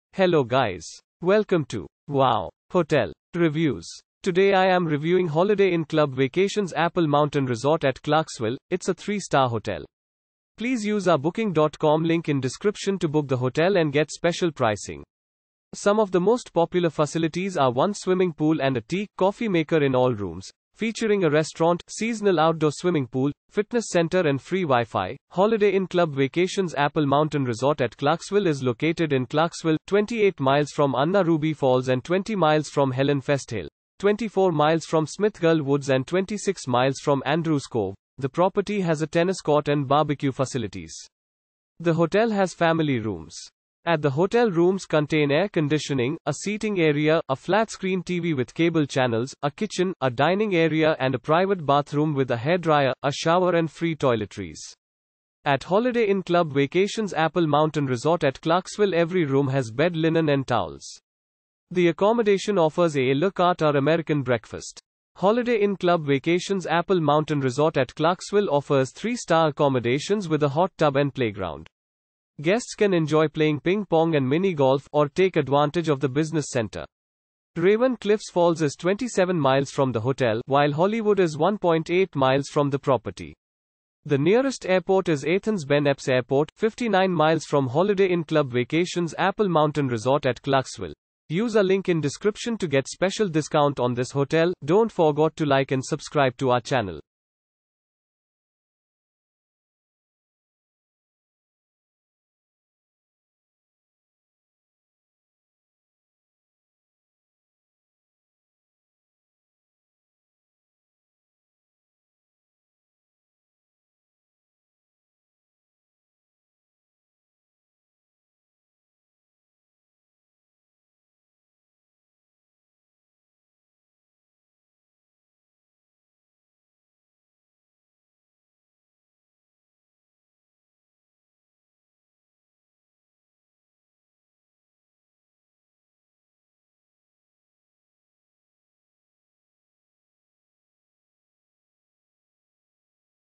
Holiday Inn Club Vacations Apple Mountain Resort at Clarkesville Review - Clarkesville , United Stat
Holiday Inn Club Vacations Apple Mountain Resort at Clarkesville

Featuring a restaurant, seasonal outdoor swimming pool, fitness center and free WiFi, Holiday Inn Club Vacations Apple Mountain Resort at Clarkesville is located in Clarkesville, 28 miles from Anna Ruby Falls and 20 miles from Helen Festhalle. 24 miles from Smithgall Woods and 26 miles from Andrews Cove, the property has a tennis court and barbecue facilities. The hotel has family rooms.
At the hotel rooms contain air conditioning, a seating area, a flat-screen TV with cable channels, a kitchen, a dining area and a private bathroom with a hairdryer, a shower and free toiletries. At Holiday Inn Club Vacations Apple Mountain Resort at Clarkesville every room has bed linen and towels.
The accommodation offers a à la carte or American breakfast.
Holiday Inn Club Vacations Apple Mountain Resort at Clarkesville offers 3-star accommodations with a hot tub and playground.
Guests can enjoy playing ping pong and minigolf, or take advantage of the business center.
Raven Cliffs Falls is 27 miles from the hotel, while Hollywood is 1.8 miles from the property. The nearest airport is Athens-Ben Epps Airport, 59 miles from Holiday Inn Club Vacations Apple Mountain Resort at Clarkesville.